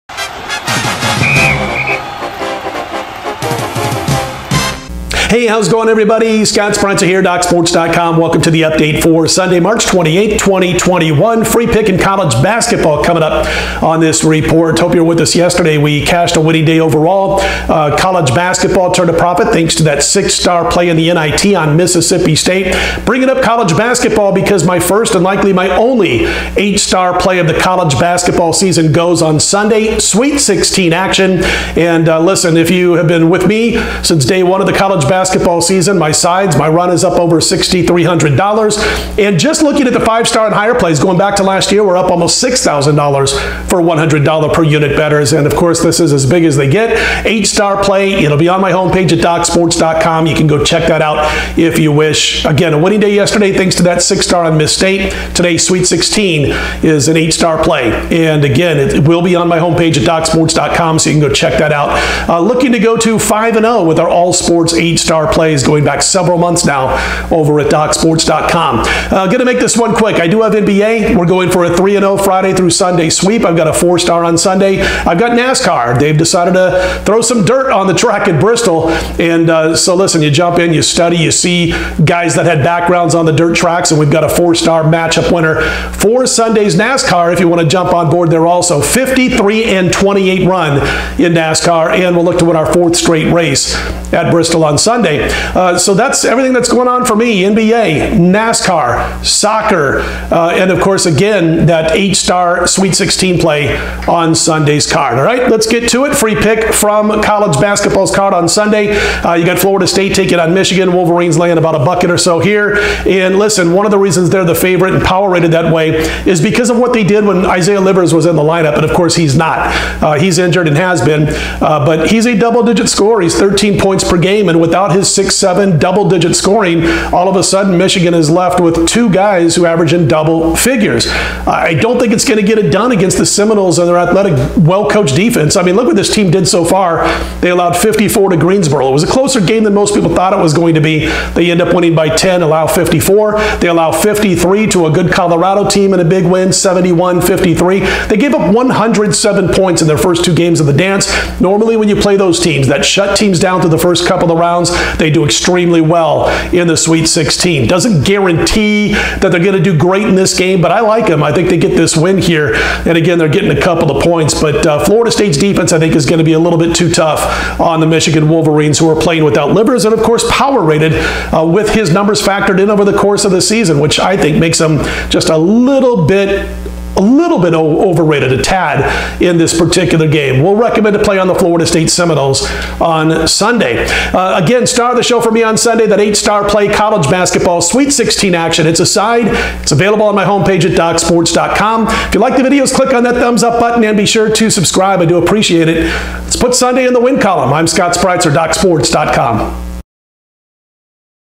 Florida State Seminoles vs Michigan Wolverines Prediction, 3/28/2021 College Basketball Free Pick
Florida State vs Michigan Prediction 3/28/21. The Florida State Seminoles travel to Indianapolis, IN to face the Michigan Wolverines at 5:00PM EST at the Bankers Life Fieldhouse.